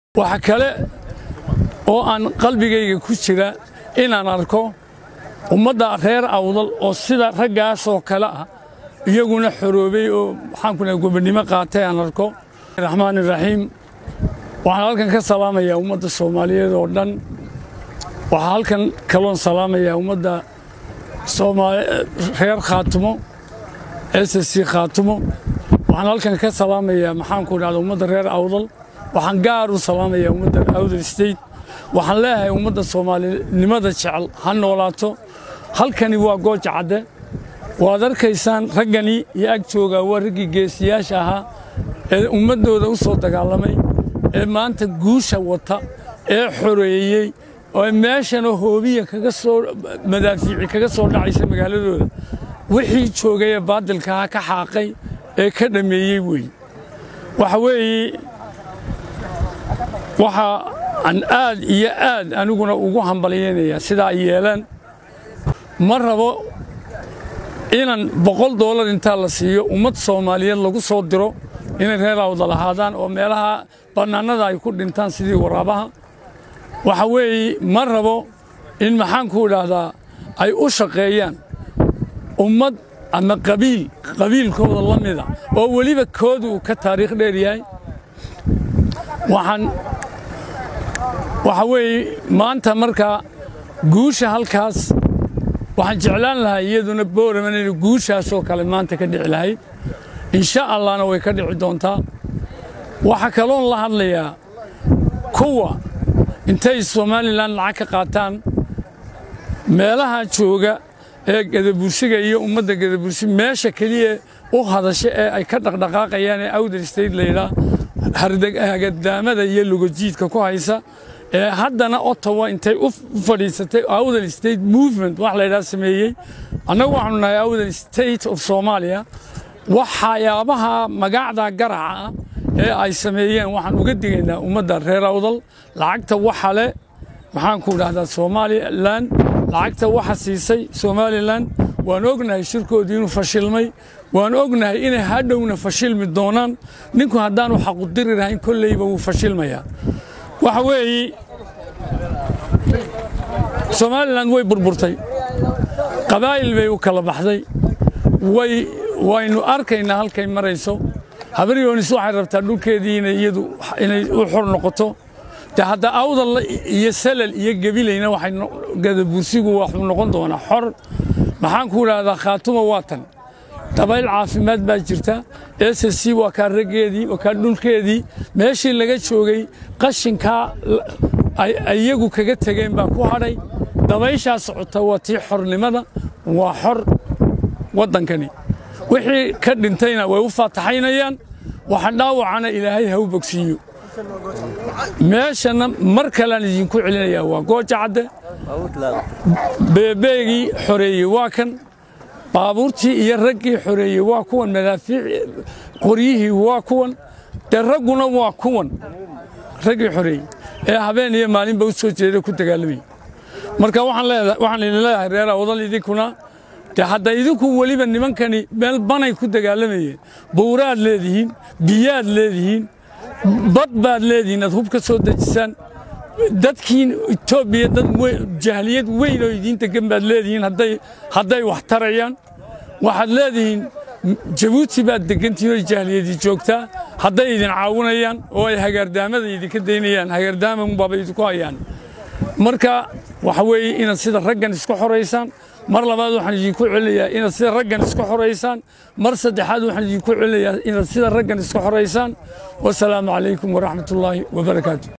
Fariin ku socota reer Awdal mar uu joogey gooja Cade Madaxweynaha Awdal State Prof Nageeye
Madaxweynaha Awdal State ayaa mudadaanba booqasho shaqo ah ku joogey maamulka SCC Laascanood, fariin mug weyn ayuu uga soo direy Gooja Cade